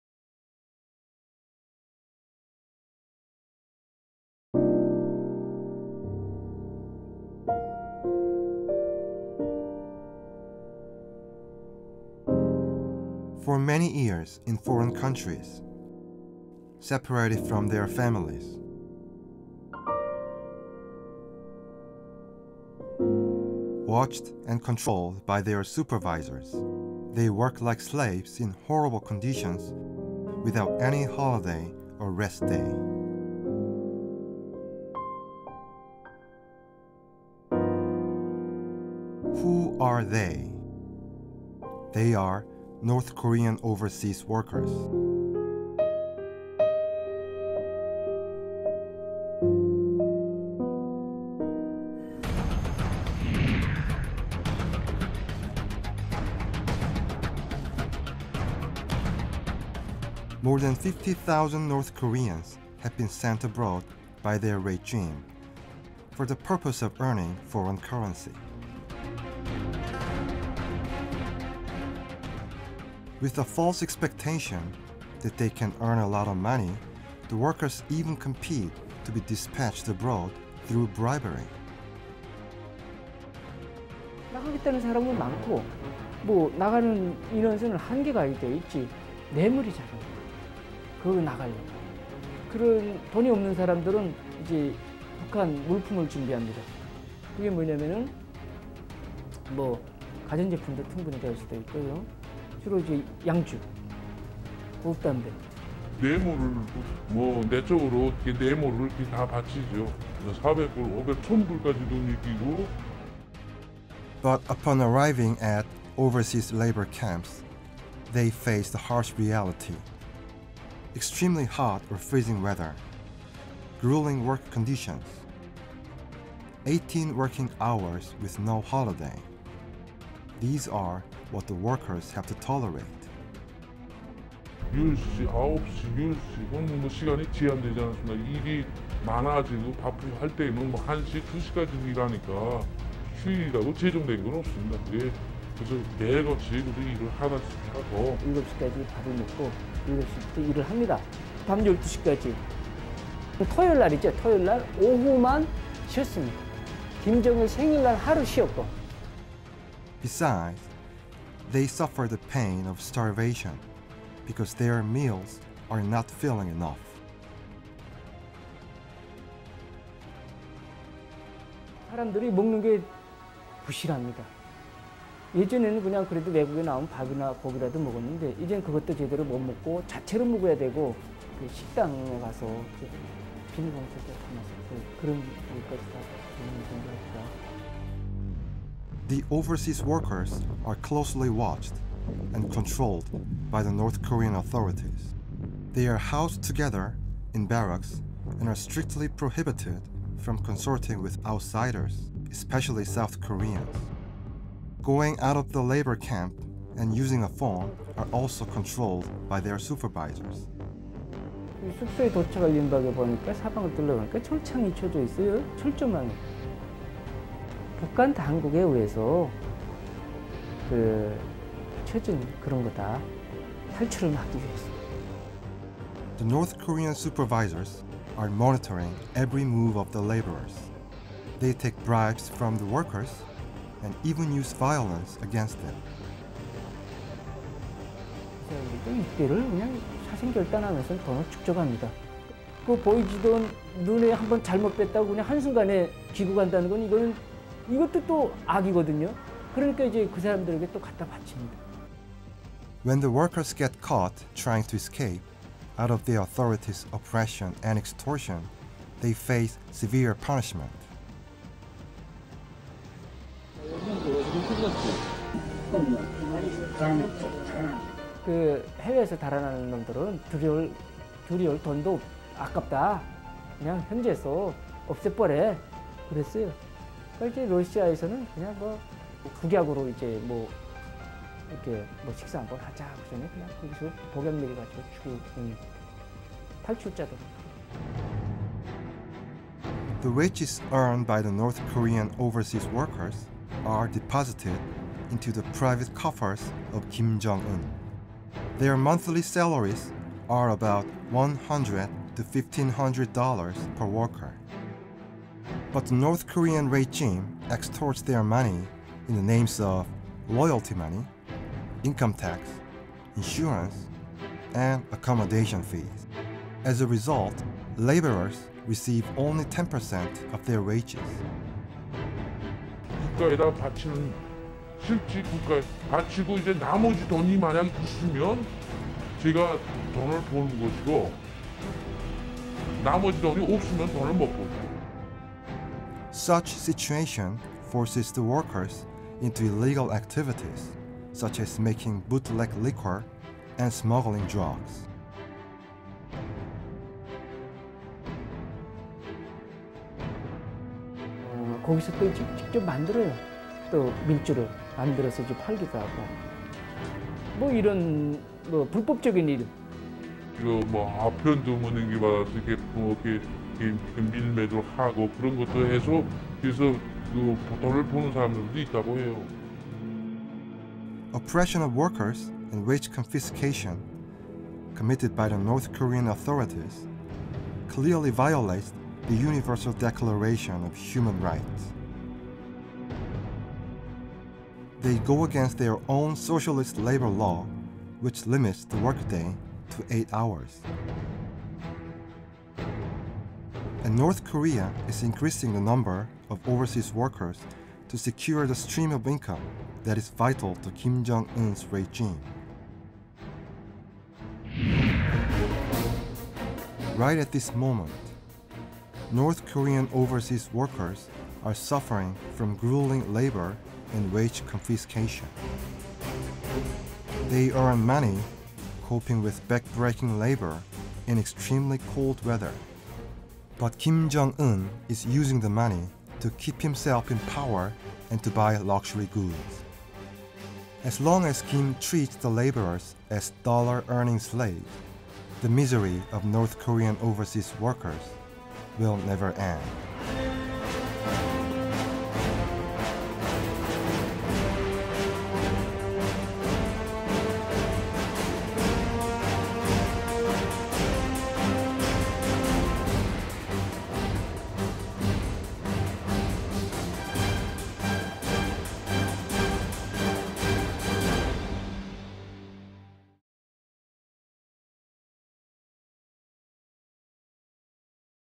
The North Korean Overseas Workers
More than 50,000 North Korean workers have been sent to over 40 countries as laborers. The dollar earning slaves work in harsh conditions including  starvation, long working hours and no holidays and only get 10% of their wages as 90% is taken by North Korean Regime.

  The Universal Declaration of Human Rights (Article 23)
Everyone has the right to just and favorable conditions of work.